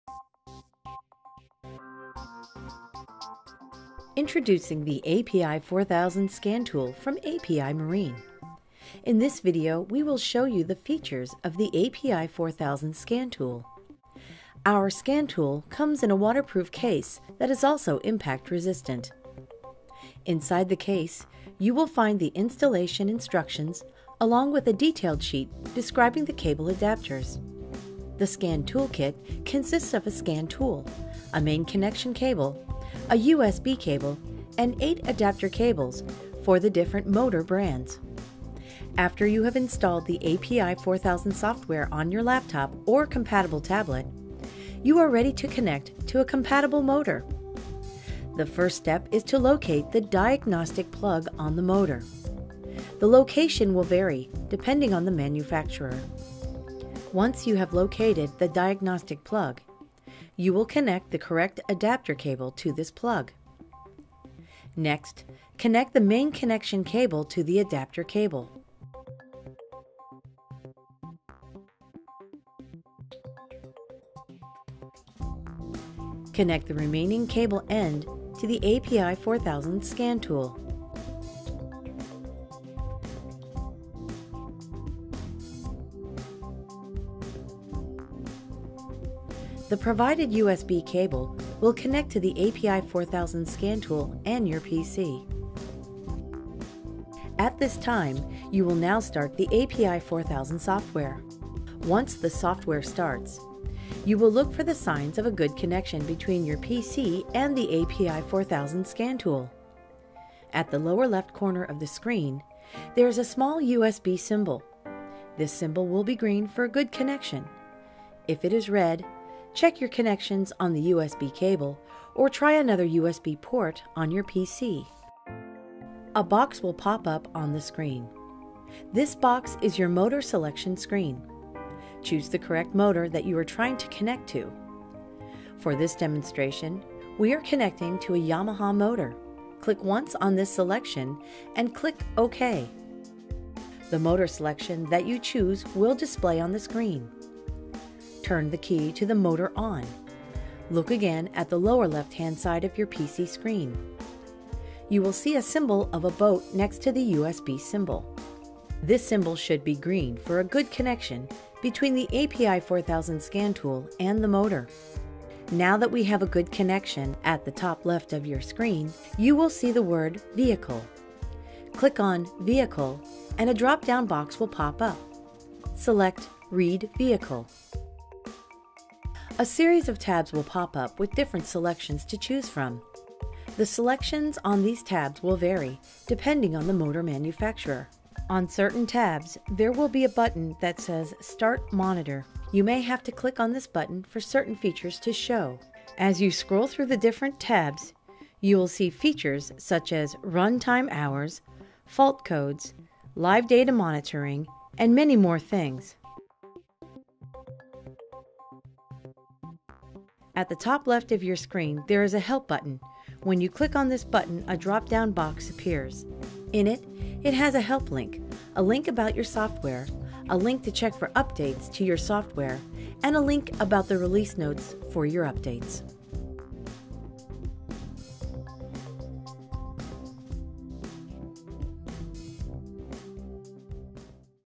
API 4000 Scan Tool
A short video describing our new API 4000 Marine Diagnostic Tool